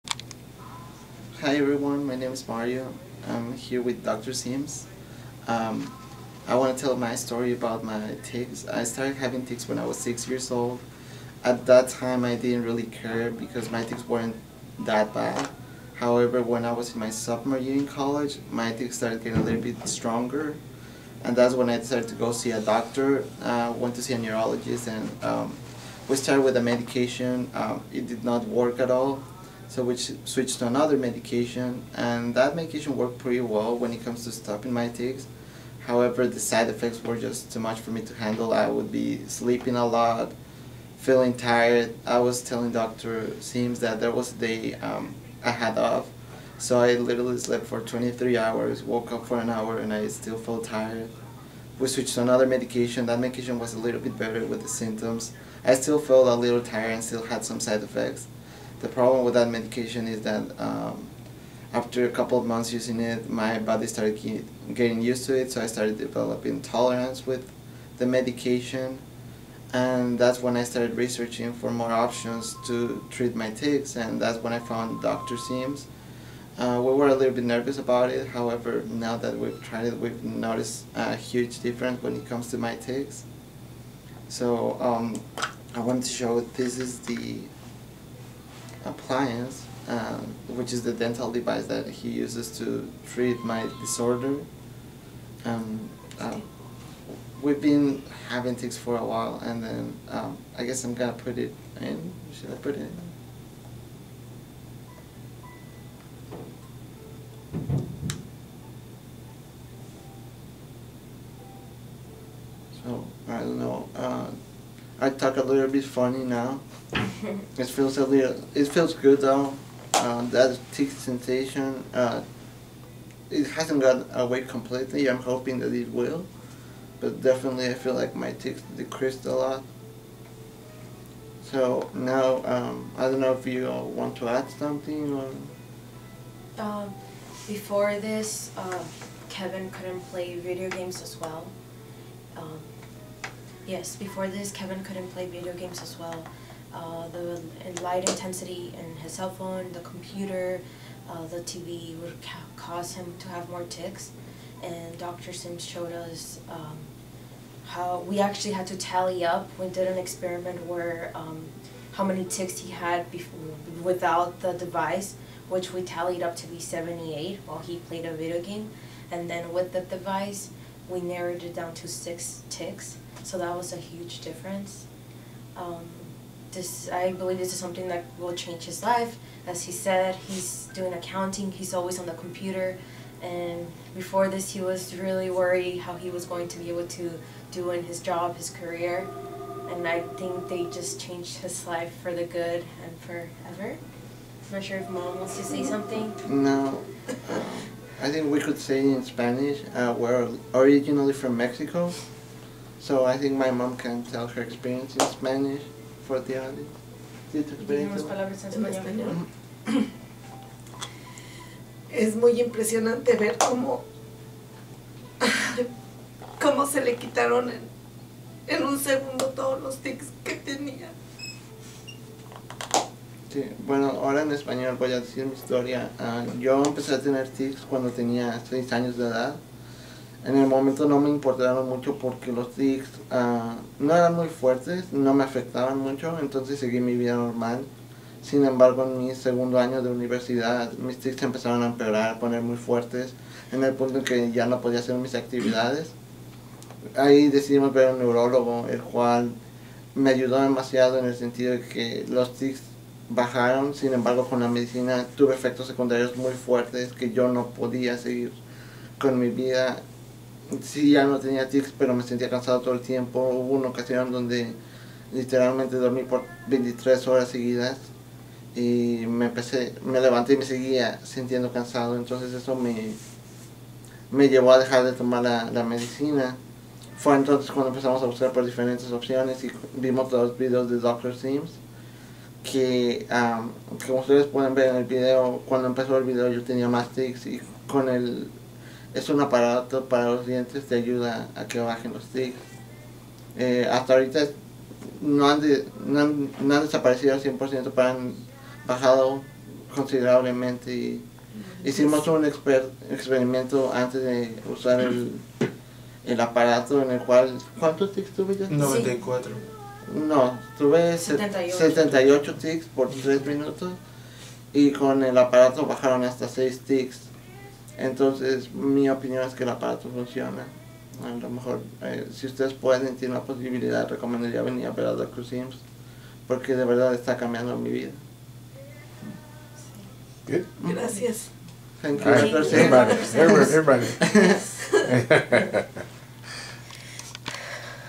Tourette's goes down!!!!
Another wonderful family came to Dr. Sims for a custom orthotic to reduce and/or eliminate the tics associated with Tourette syndrome. Here is his video.